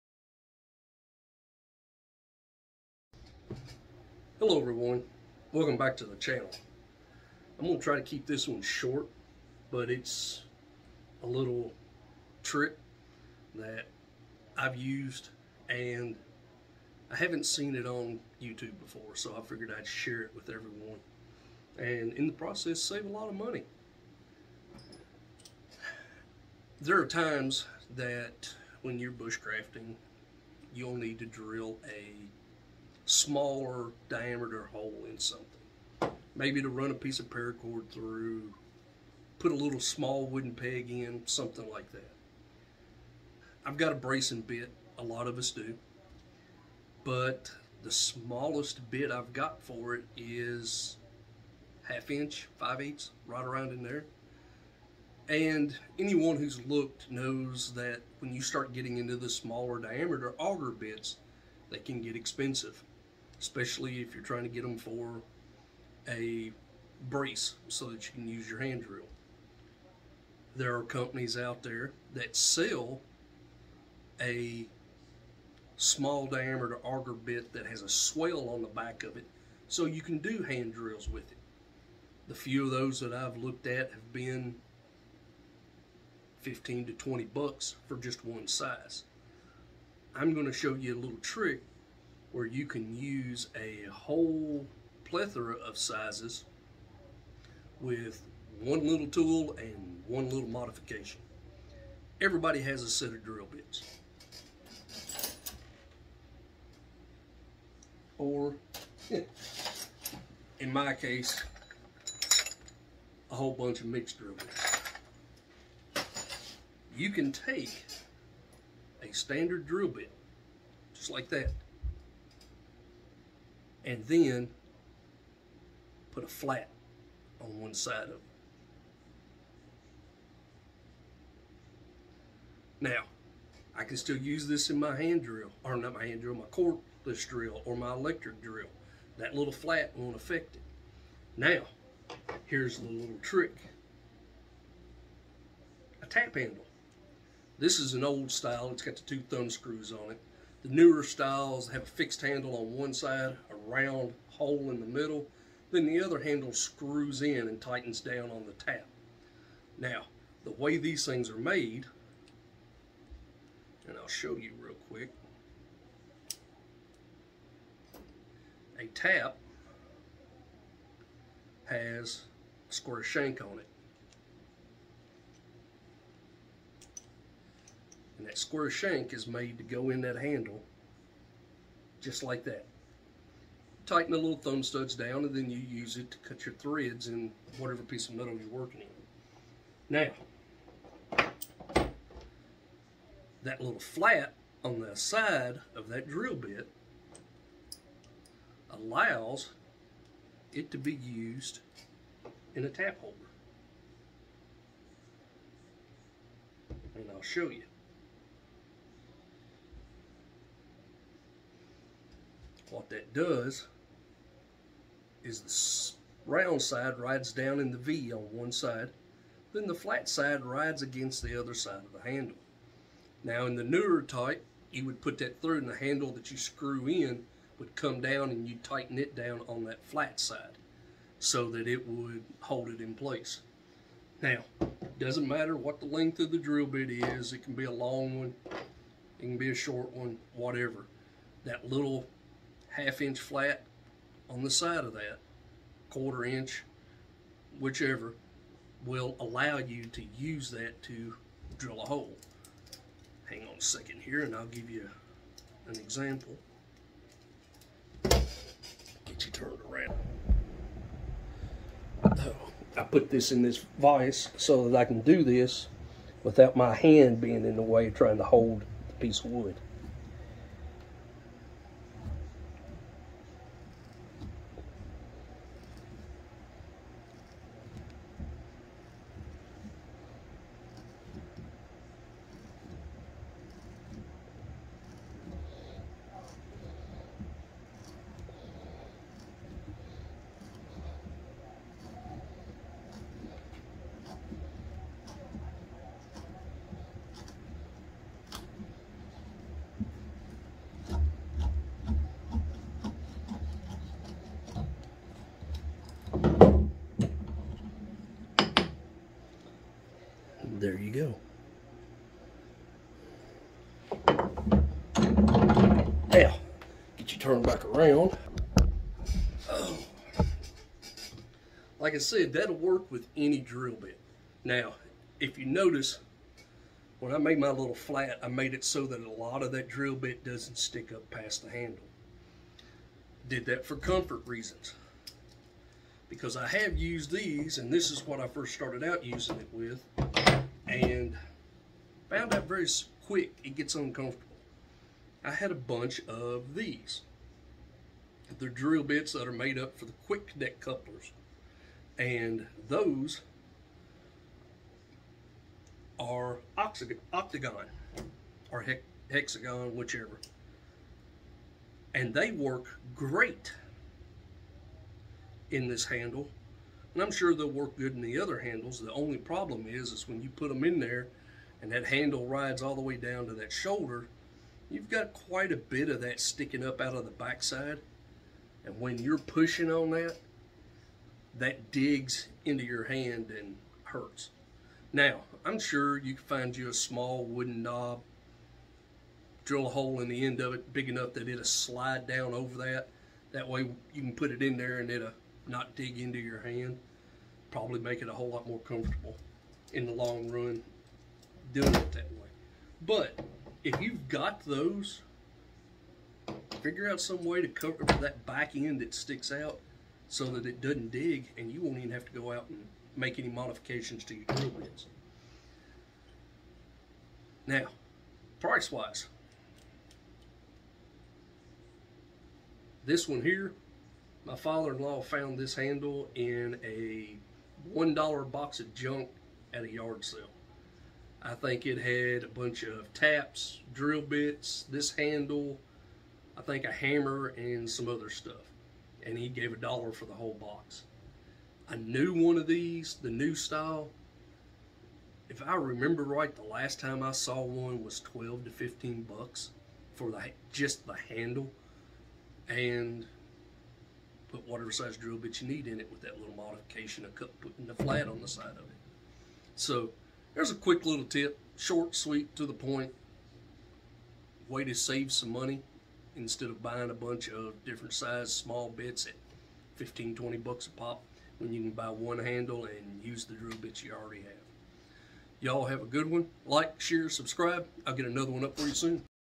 Cheap Bushcraft Hand Drill
Here's a cheap trick for a bushcraft hand drill! Never seen this on Youtube so I wanted to share.